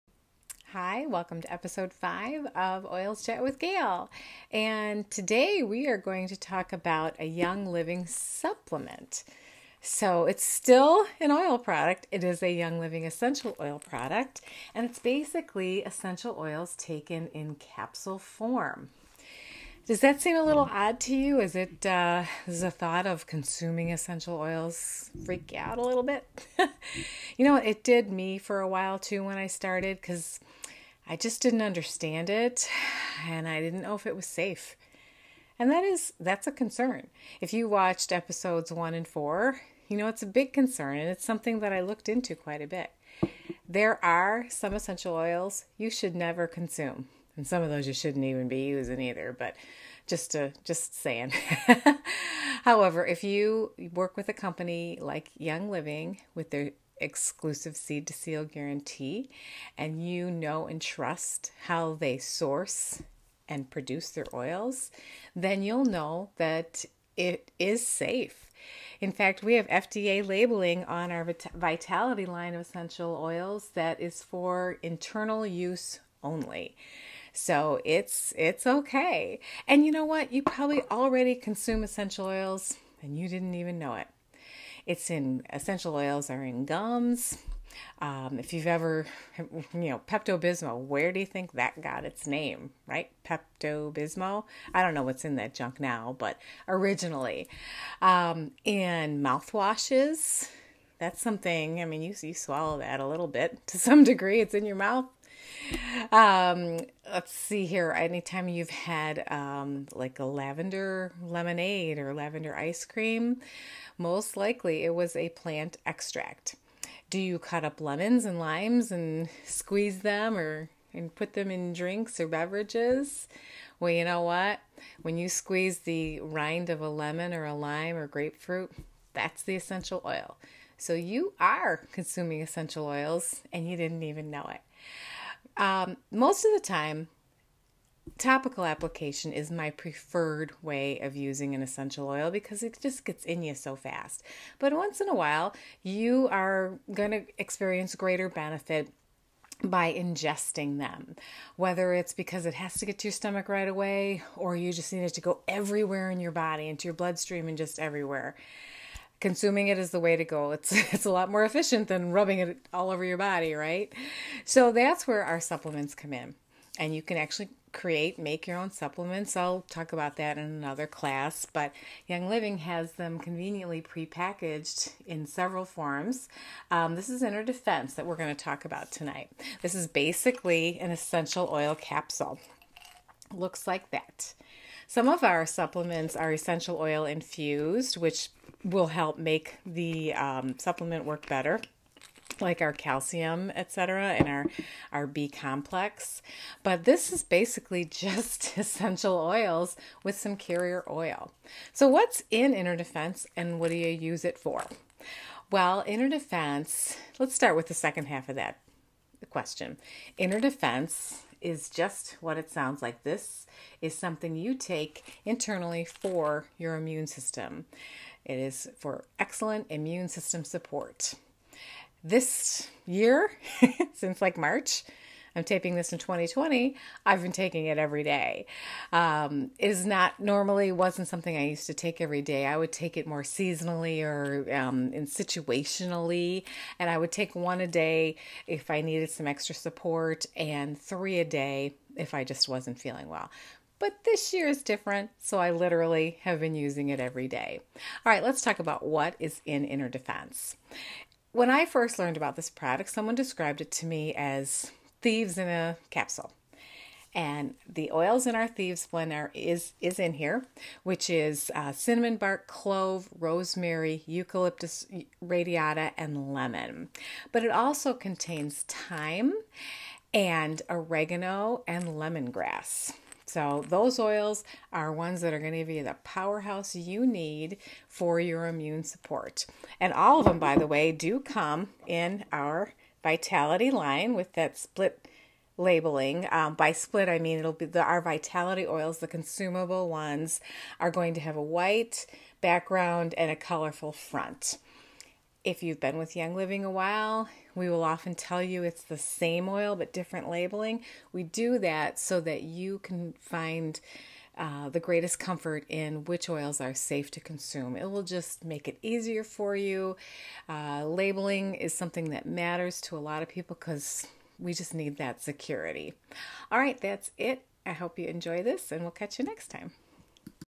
Oil Chat with Gayle, Episode 5: Inner Defense
In Episode 5, let's talk about an important supplement no must NOT be without, especially during fall, winter and spring!  It's Inner Dense, and it's perfectly named!  Let's learn more!